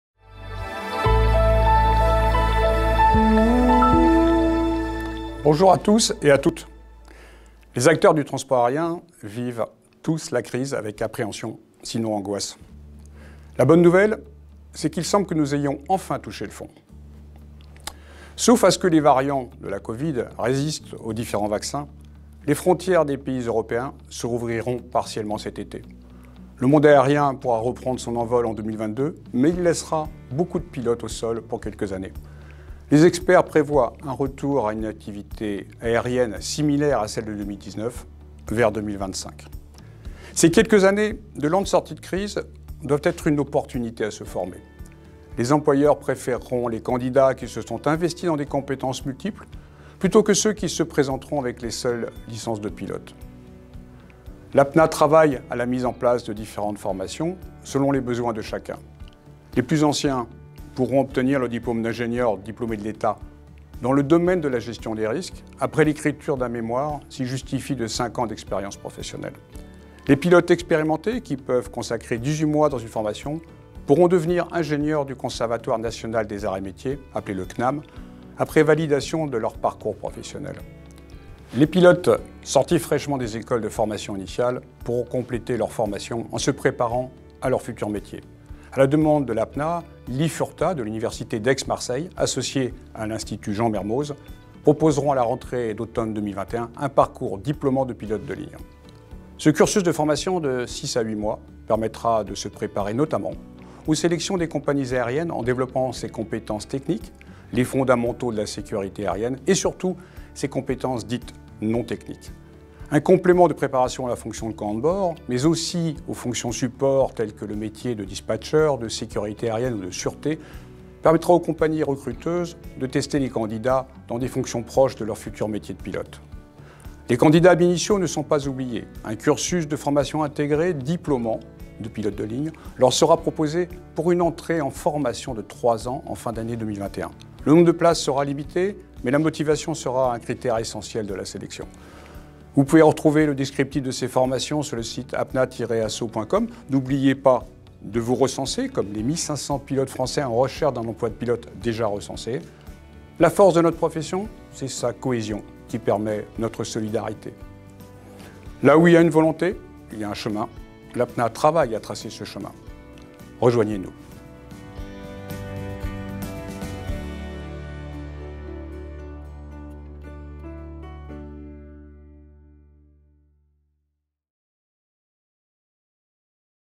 Formations APNA: Reconversion Ingénieur Pilote et Formations Diplômantes
Pilotes, diversifiez-vous avec un diplôme d'ingénieur ! 👷🏻🎓

L’APNA en partenariat avec le CNAM, la SIDPE et l’assistance de l’APEC vous propose 2 types de formations d’ingénieur :
1️⃣ Ingénieur diplômé par l’état (IDPE)
2️⃣ Ingénieur du Conservatoire national des métiers (CNAM).

Par ailleurs, à l’initiative de l’APNA, l’IFURTA de l’Université d’Aix-Marseille, en coopération avec l’institut Mermoz, proposent une formation diplômante de pilote de ligne à destination, soit des ab initio sur 30 mois, soit des stagiaires sortis des ATO sur 6 mois, préparant aux compétences requises par les compagnies aériennes

C’est en période de crise qu’il faut se former, pour être prêt à répondre aux exigences des sélections, lors de la reprise. 🚀

La sélection aura lieu en juin, pour une entrée limitée à 25 stagiaires dans chaque cursus, pour une entrée à l’automne 2021.

Les pilotes intéressés par cette formation peuvent remplir le formulaire en ligne suivant :

Pour plus d'informations, contactez-nous sur: *